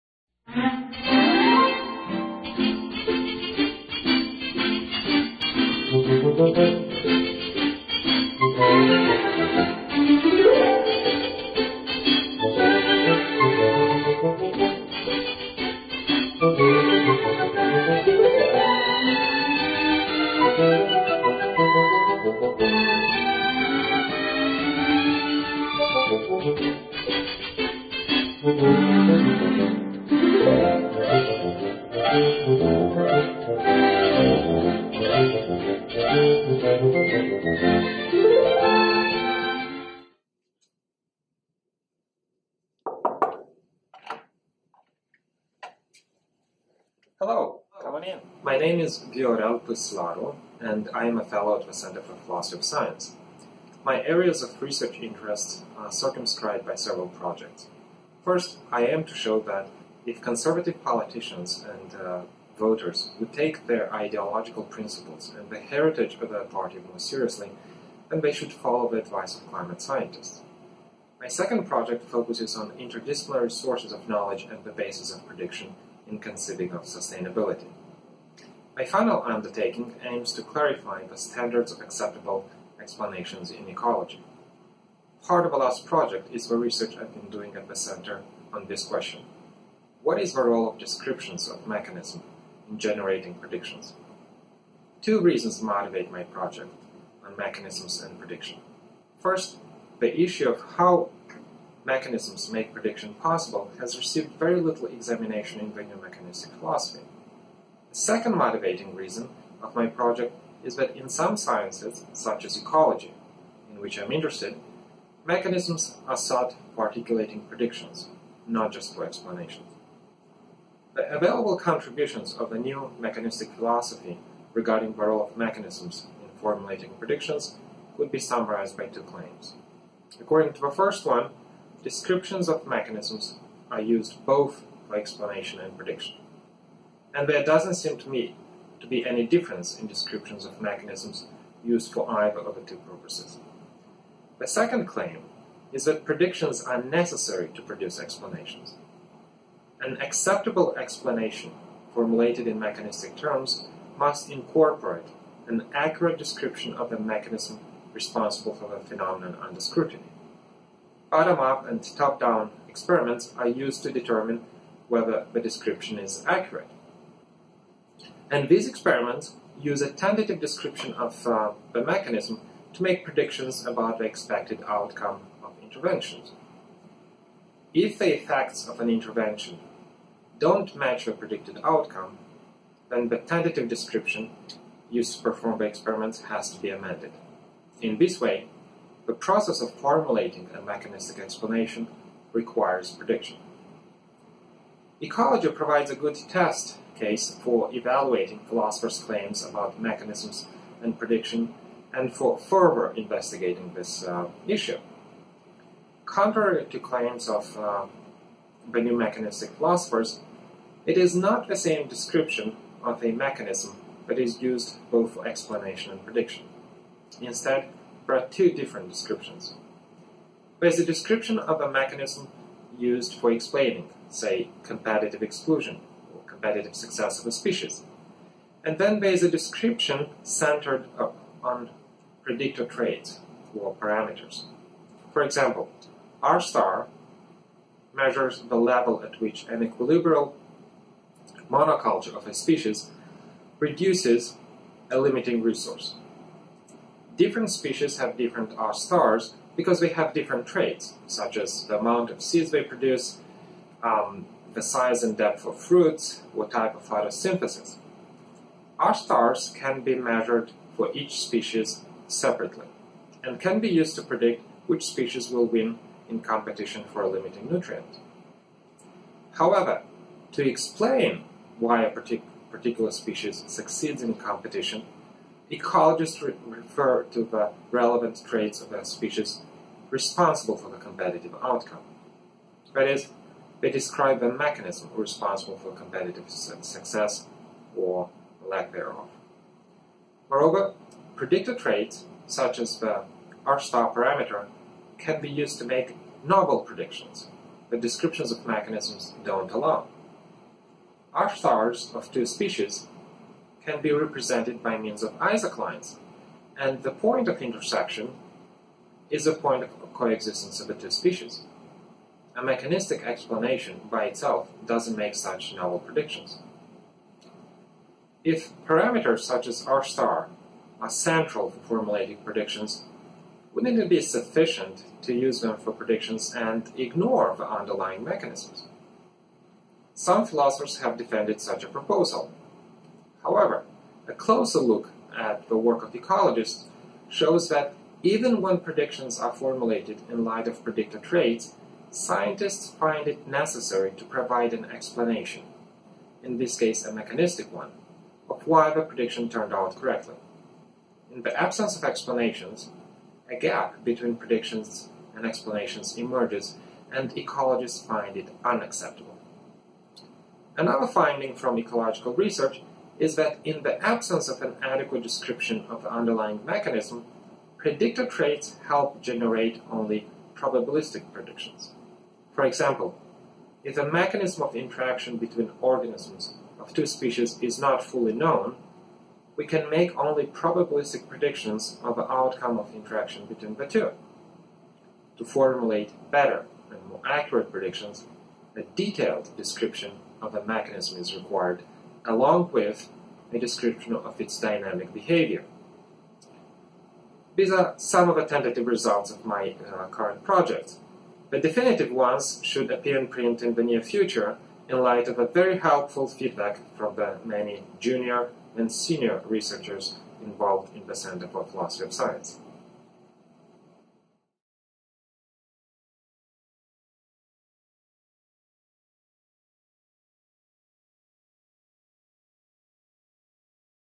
5-Minute Fellows: Viorel Pâslaru
Viorel Pâslaru from the University of Dayton is a Fellow with us this Fall.  Here he talks about his work with prediction, mechanisms, and explanation, and the role they play in relation to ecology.